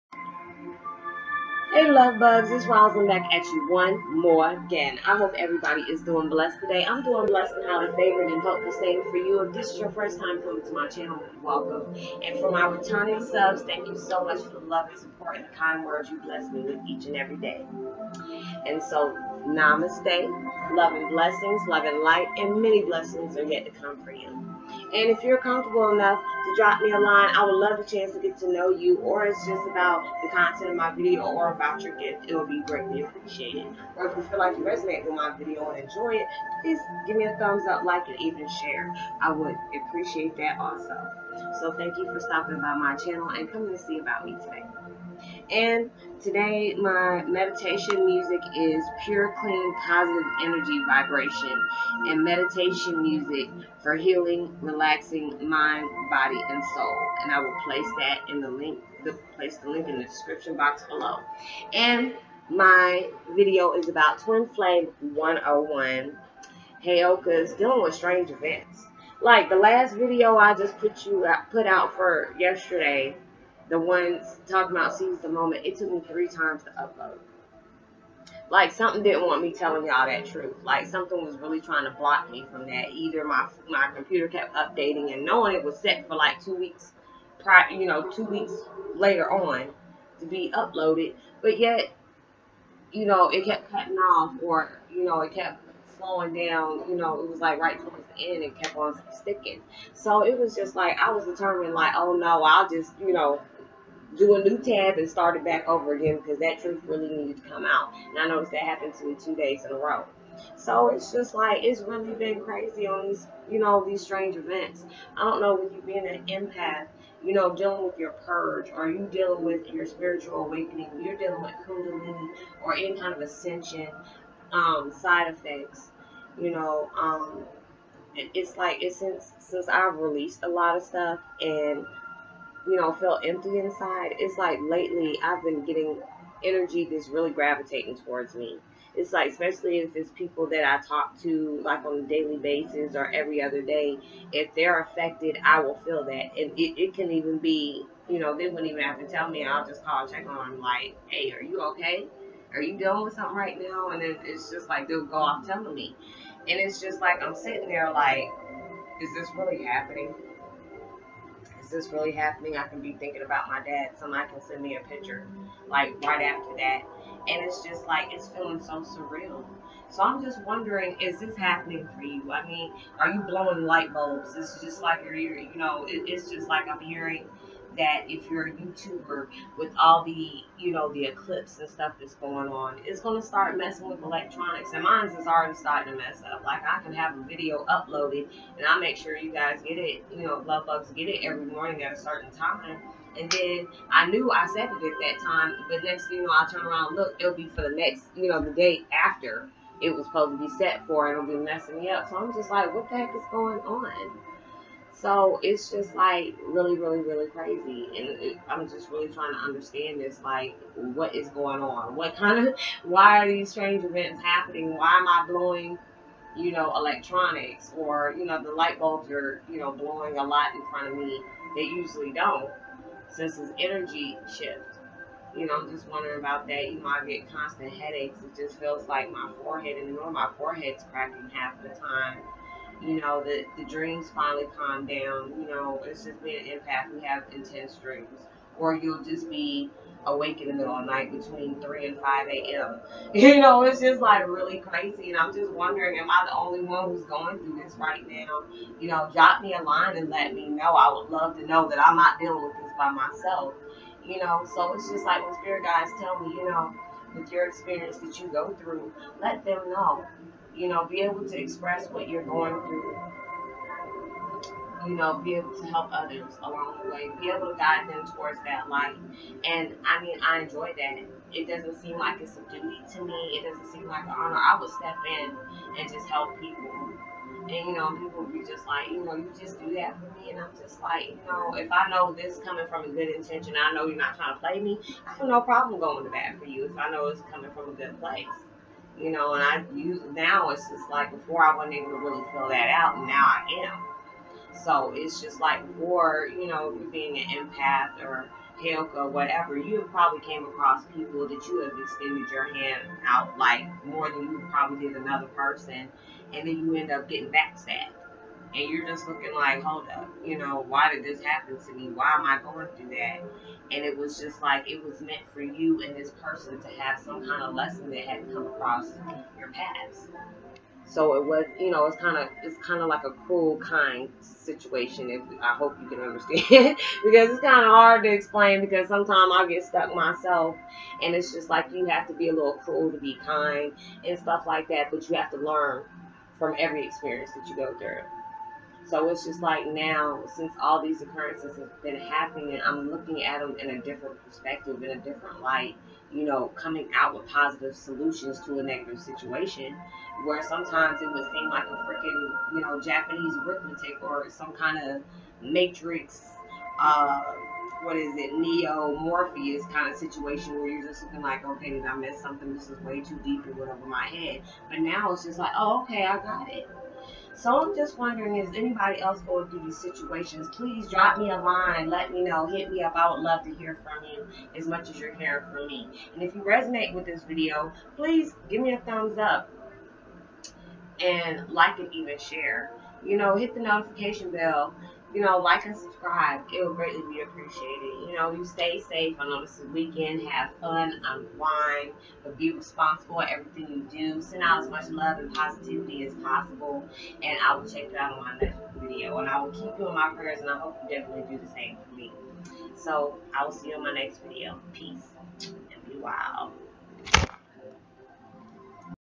Heyoka's Dealing With Strange Events
Ever since my Past Hurt Purge, strange events have been happening and wondering if anyone else is going thru too